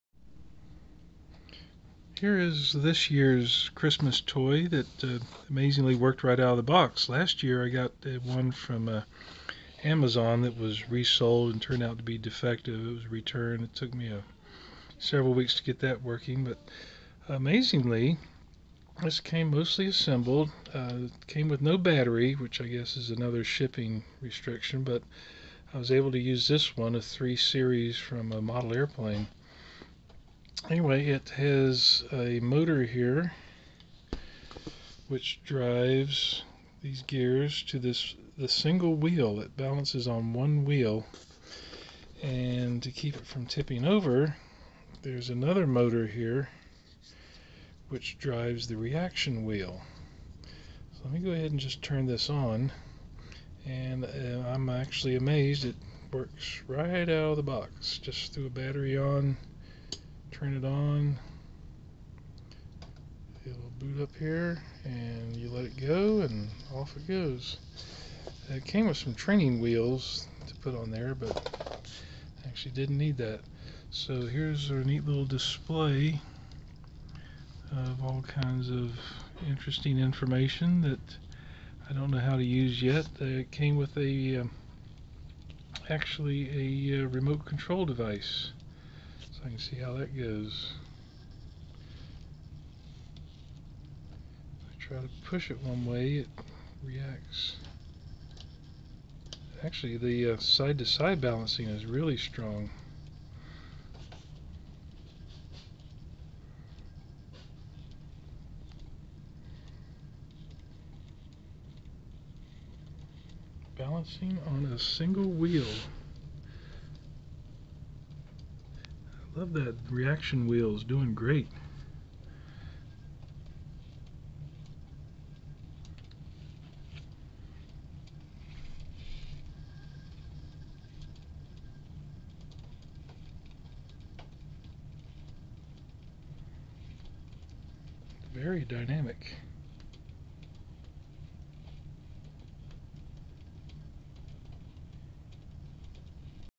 Unicycle robot balances on a single wheel using a reaction wheel
balance on single wheel with reaction wheel - stands upright.